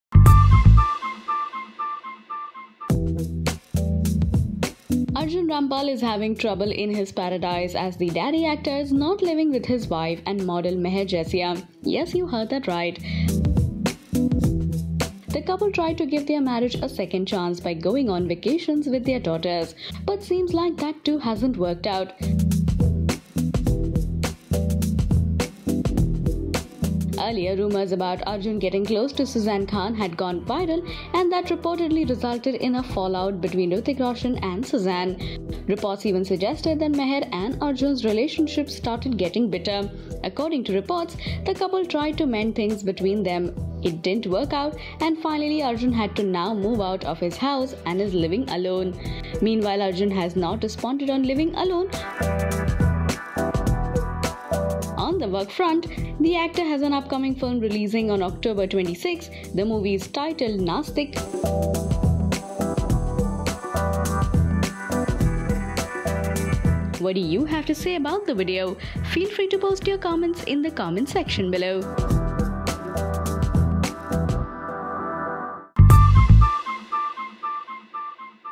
SHOCKING: Arjun Rampal And Mehr Jessia To Get Divorced
Hush-hush whispers claim that the Roy actor and his model wife have decided to file for divorce in